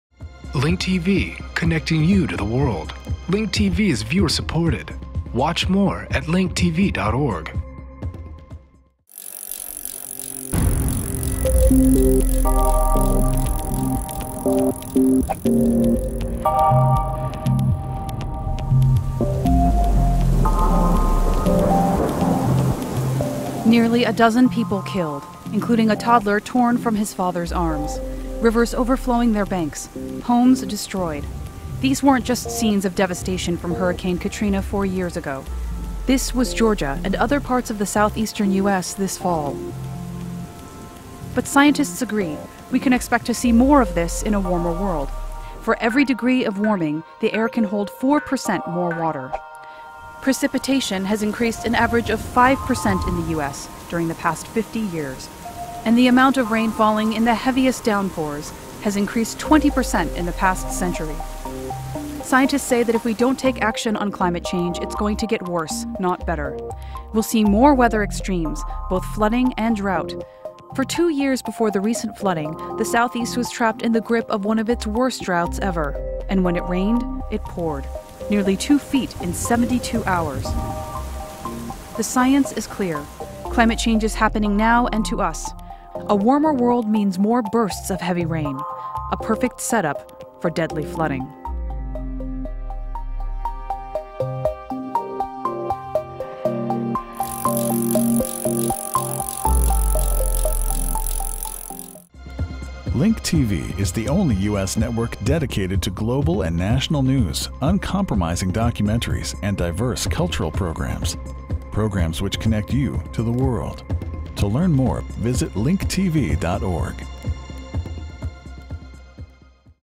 Climate Change Hits Home: Georgia
This climate change factoid portrays the intensifying power of weather in the southern United States. Only weeks ago, Georgia was buried in water when hit by a fluke storm that killed almost a dozen people. Scientists say weather this extreme is becoming the norm, due to rising global temperatures.